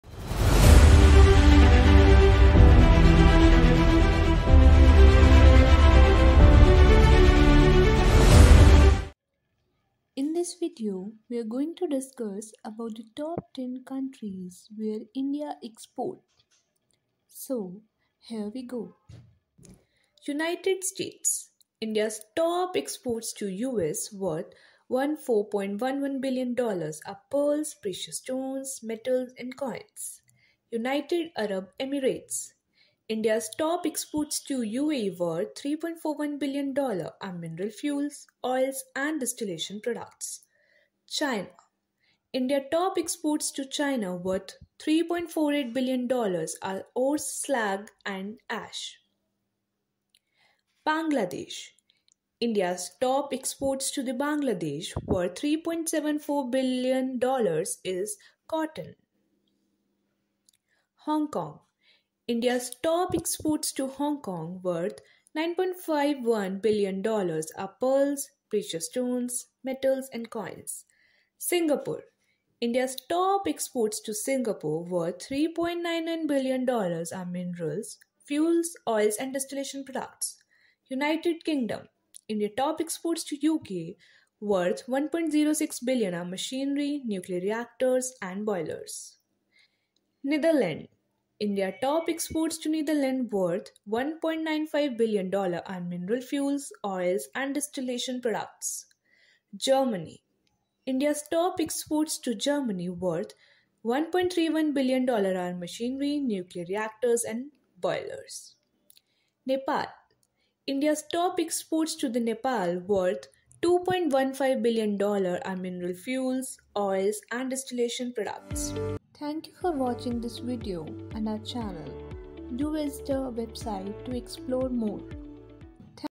Top 10 countries where India export
India export to many different countries. In this video, you can see the top countries where India export. How much value of exports to the particular country? What are the particular items that are known to be exported to the particular country?
This video will give all the necessary information.

𝗝𝗼𝗶𝗻 𝗚𝗹𝗼𝗯𝗮𝗹 𝗧𝗿𝗮𝗱𝗲 𝗣𝗹𝗮𝘇𝗮 𝘁𝗼𝗱𝗮𝘆 𝗼𝗿 𝗴𝗶𝘃𝗲 𝘂𝘀 𝗮 𝗰𝗮𝗹𝗹 𝗮𝘁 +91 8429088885.